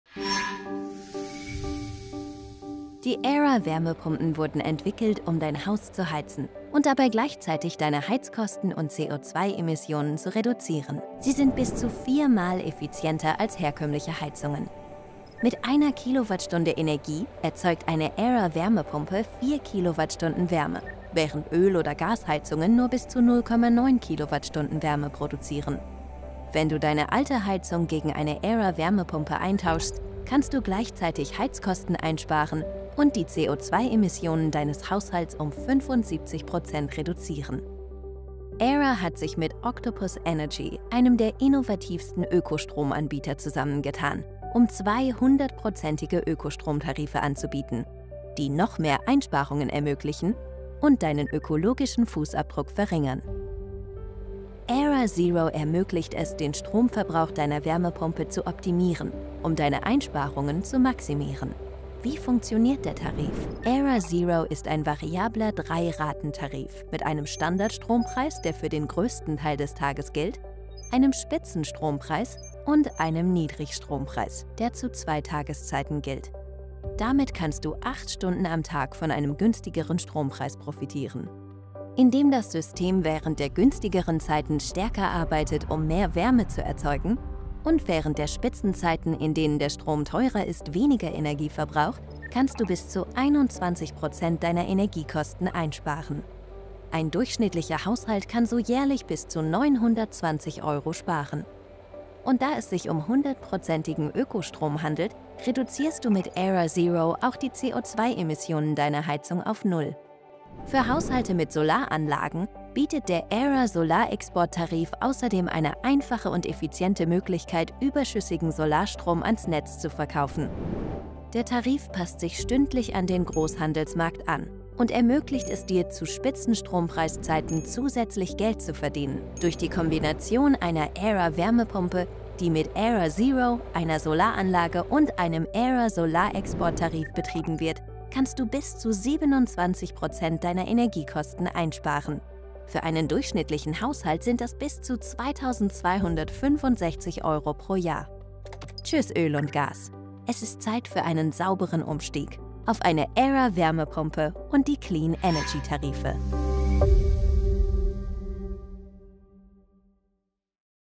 Aira Clean Energy Tarife
Wir stellen Ihnen Aira Zero vor: einen 100%-igen Ökostromtarif, der es Aira ermöglicht, den Energieverbrauch Ihrer Wärmepumpe zu optimieren, um noch größere Einsparungen zu erzielen und die Umweltbelastung zu reduzieren.